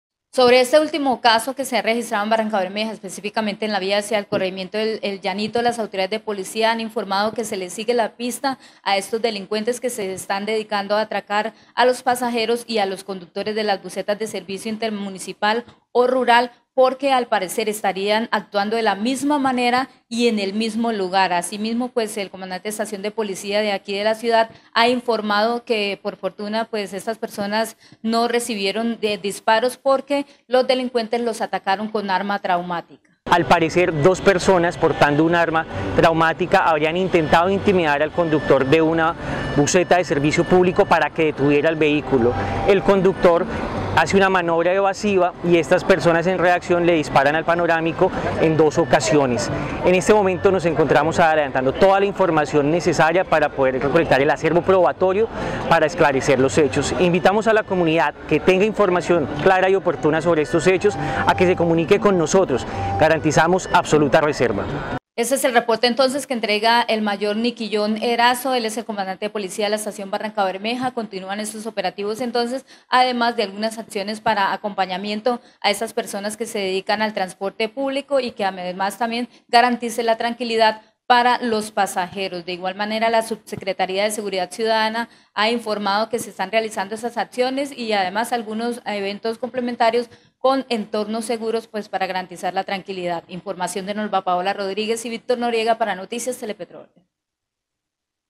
"Delincuentes dispararon con arma traumática": Policía
Noticias Telepetróleo consulto a las autoridades de policía sobre las acciones que se están implementando para proteger la vida de los conductores de busetas de servicio intermunicipal. veamos el informe de Nolva Rodríguez.